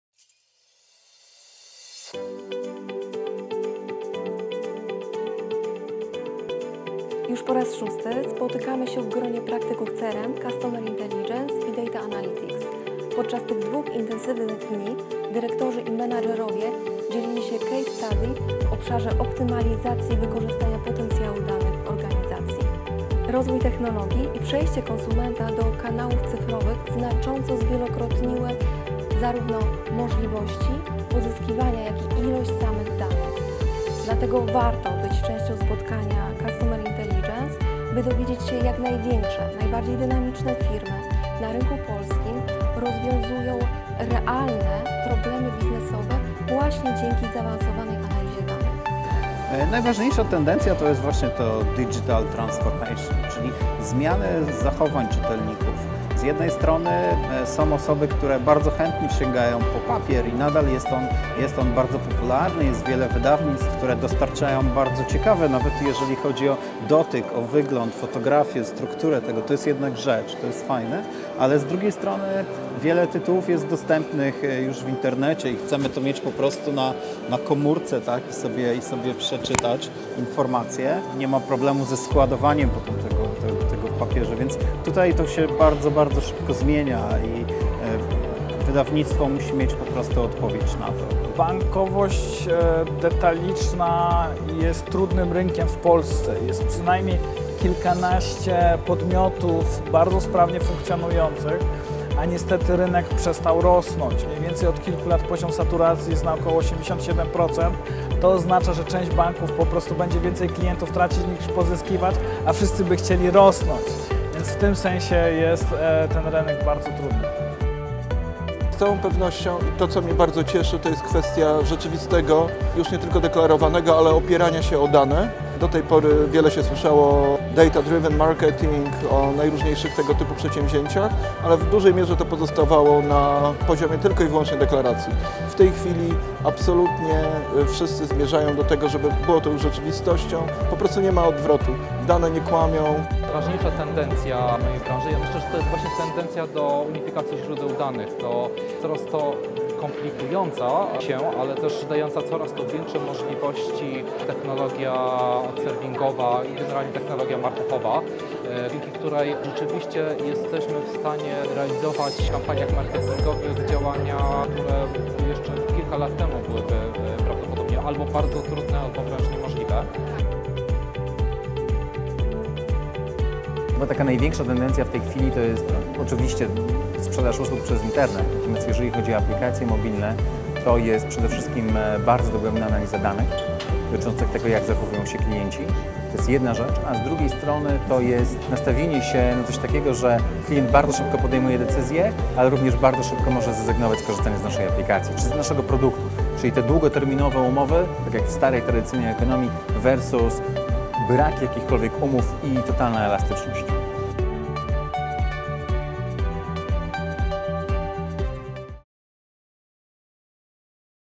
VI Customer Omnidata
Forum Customer OmniData to spotkanie praktyków CRM, Customer Intelligence i Data Analytics. Celem wydarzenia jest zaprezentowanie best practices w obszarze optymalizacji wykorzystania potencjału danych klientów w organizacji - od etapu ich zbierania i integrowania poprzez analizę aż do tworzenia zestawów informacji do wykorzystania przez biznes.